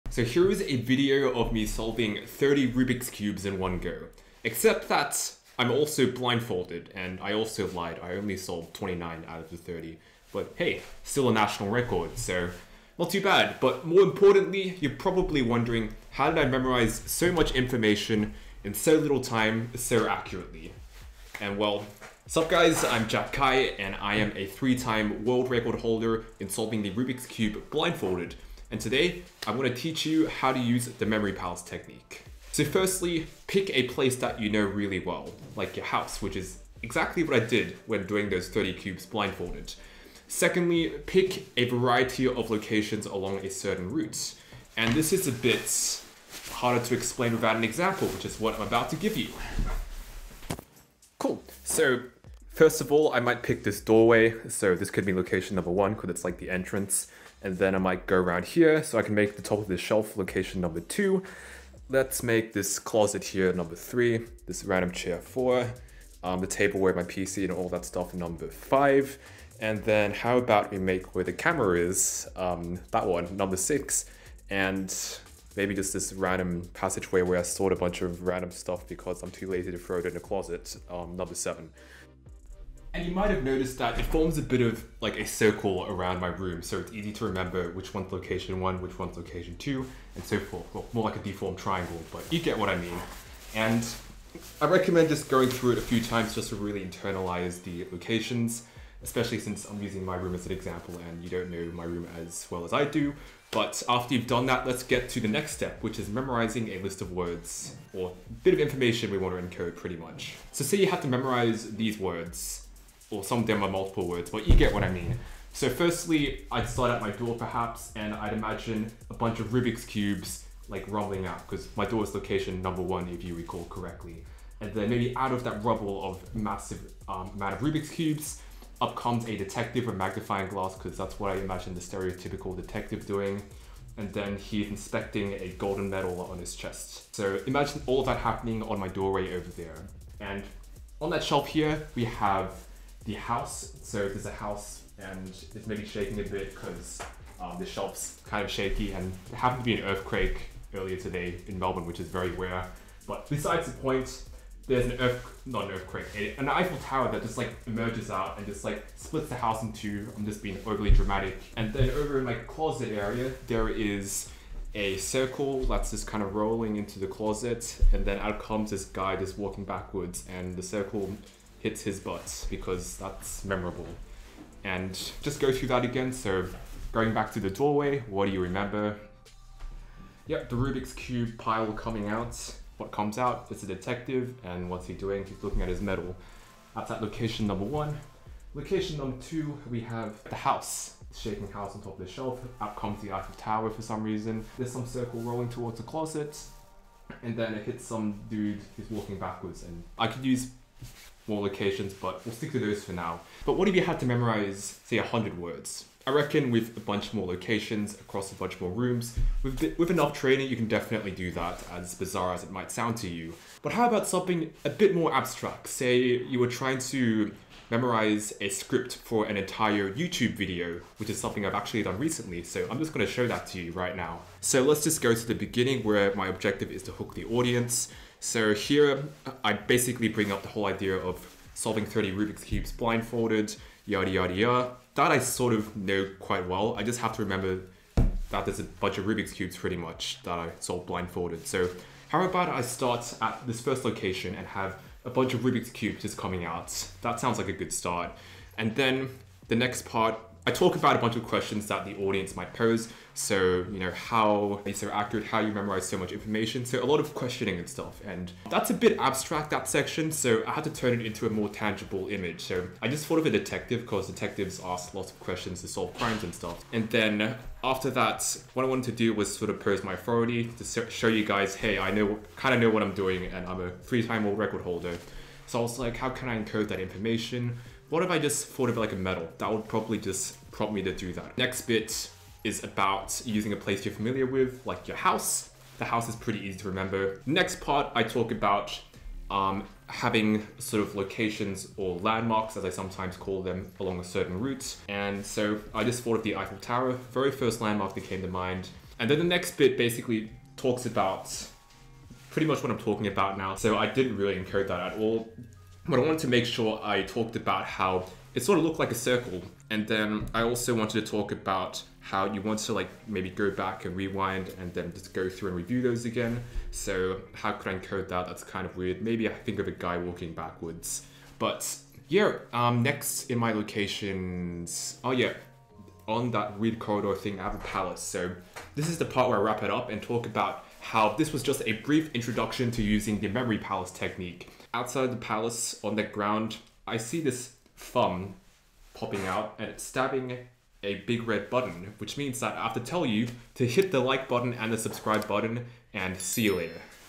How to Memorize Anything Using the Memory Palace Method
Wondering how to memorize large amounts of information and recall them quickly? Then you gotta learn how to use the memory palace technique, sometimes also known as romans rooms/the journey method.

I've used the same method to break a national record in solving 29 out of 30 Rubik's cubes blindfolded (in one go) in less than an hour and have started figuring out ways to utilise it in other areas of life so thought I'd share 👍

Also I don't often make videos aimed at a more general audience (outside of me solving Rubik's cubes blindfolded really fast) so lemme know what you think!

📱 I have too many social media platforms to manage ahhhhhhh